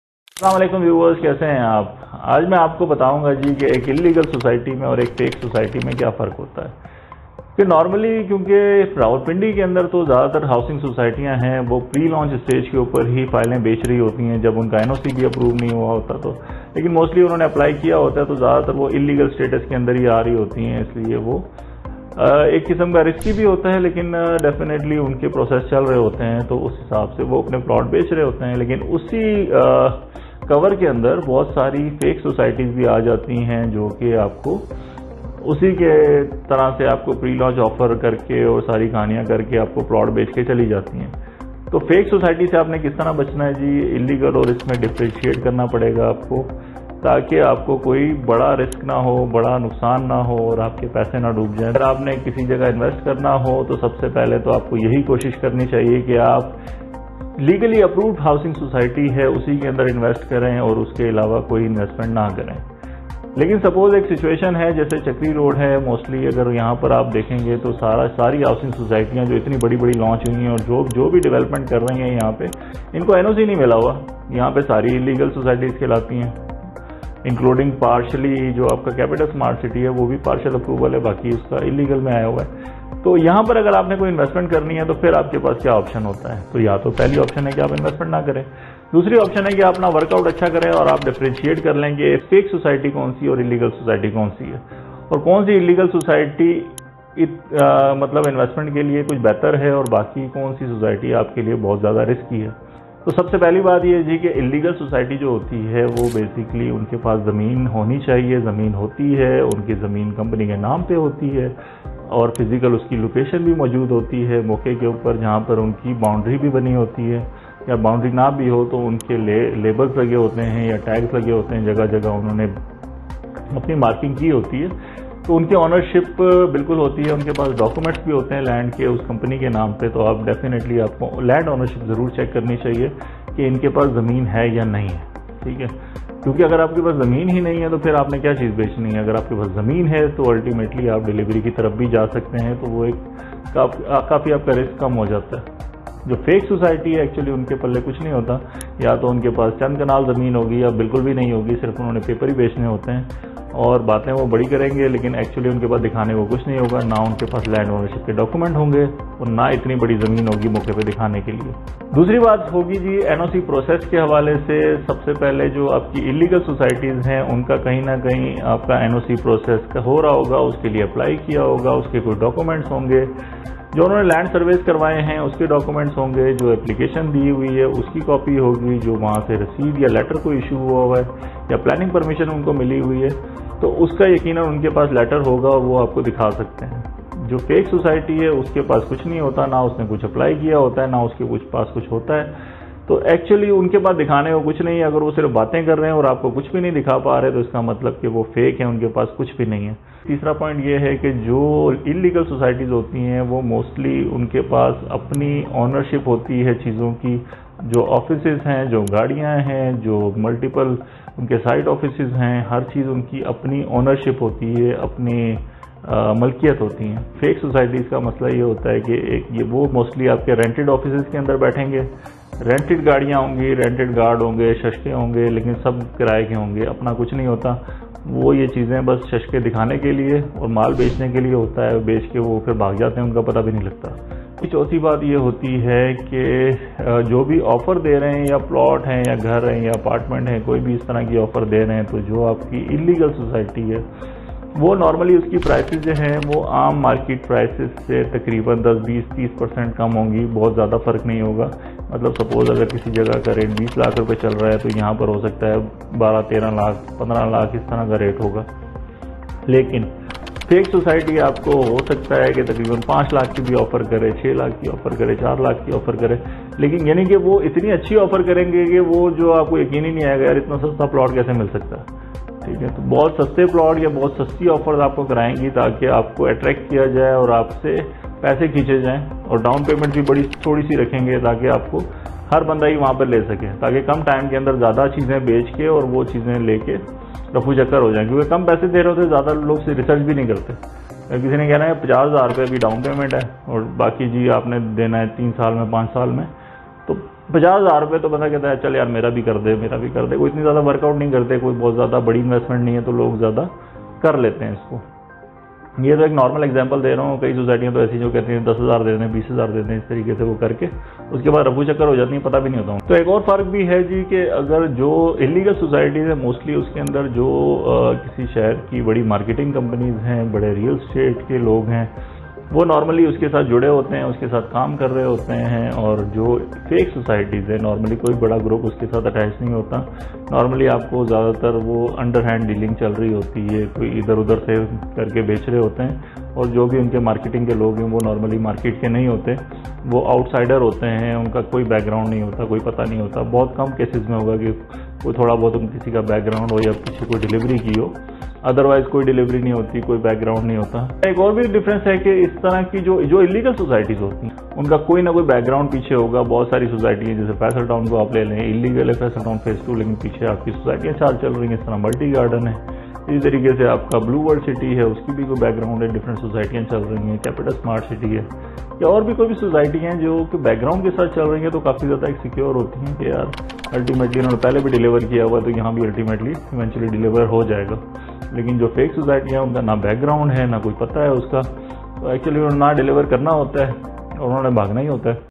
Fake Housing Societies & Illegal Housing Societies | How to avoid investing in Fake Housing Schemes?
Welcome to our detailed video on how you can save yourself and your hard-earned money from fake housing societies. As you already know, new housing projects emerge on a daily basis, especially in the twin cities. Out of hundreds, only a few projects are legal and possess a duly approved NOC from Authorities.

Most of the housing societies in Rawalpindi are selling plots at the pre-launch stage. Some of them have applied for the NOC, while others haven't. Similarly, some housing projects are under process by the authorities. Fake societies are often found in these cover-selling plots.

You should explore all the aspects of housing projects to save yourself from fake housing projects and big losses. Legal housing projects have a duly approved NOC from administrative authorities. But there are exceptions, numerous big and renowned projects are under review and don't have an NOC.

A major example is Capital Smart City, which is partially approved by RDA. There are 2 options for you, either don't invest or do your homework and deep research before investing.

First of all, check the land ownership of a project, usually legal projects have the land ownership and boundary around their land. Furthermore, there are labels and billboards indicating land ownership.

The legal and authentic project owners have ownership of different things such as vehicles, offices, site offices, investors, staff, etc. However, illegal project owners use rented offices and vehicles, etc to impress the general public.

Lastly, fake projects are not marketed by big real estate agencies. Their marketing is usually carried out by new and unknown people belonging to unclear sources.